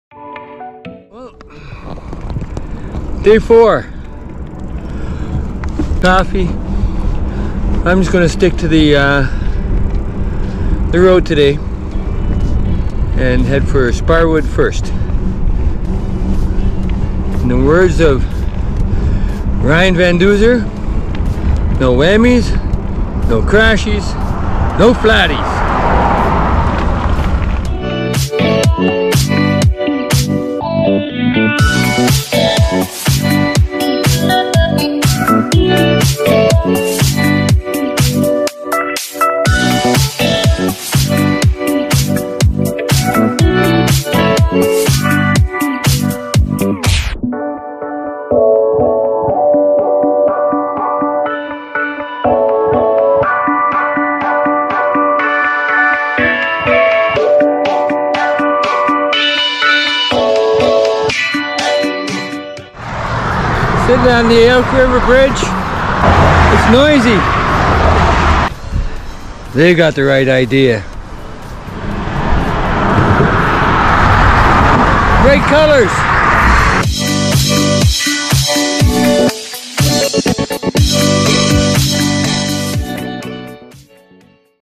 GCCC 2023 Elkford - Fernie
After a good sleep at the Elkford Community Campground, it was time to head to Sparwood, then Fernie.